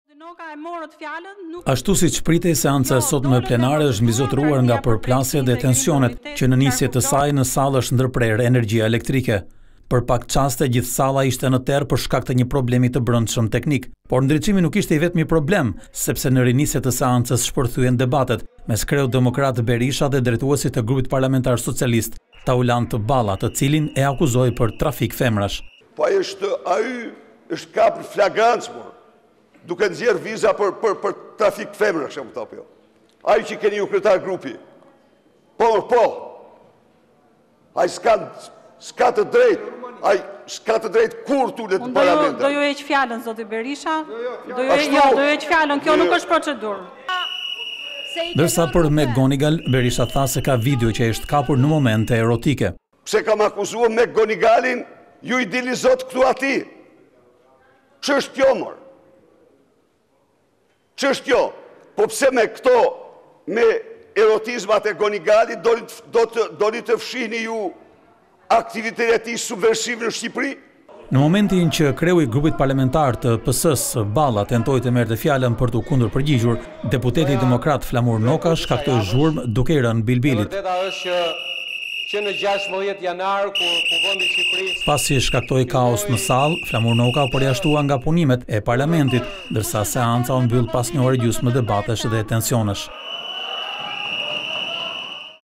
Ndërpritet energjia në Kuvend/ Zbardhet shkaku, ndërpritet seanca - Vizion Plus
Seanca e sotme plenare, ashtu siç pritej ka nisur me  incidente. Fillimisht kanë qenë disa deputetë te opozitës, ata të cilët kane tentuar të hyjnë në mjediset e brendshme te kuvendit, ndonëse janë të përjashtuar për 10 ditë.

Në nisje të fjalës së kryeparlamentares Lindita Nikolla, në sallë është ndërprerë energjia elektrike. Transmetimi live në Kuvend u ndërpre, ndërsa kanë qenë deputetët e pranishëm në seancë ata të cilët kanë “ndriçuar” ambientin me blicat e telefonave.

 Sipas Kuvendit, seanca u ndërpre pasi u dogj pajisja UPS, e cila shërben për balancimin e energjisë  në sallën e seancave plenare.

Duke marrë shkas nga kjo ndodhi, kryeparlamentarja ka ndërprerë seancën per disa minuta, deri në rikthimin e energjisë elektrike.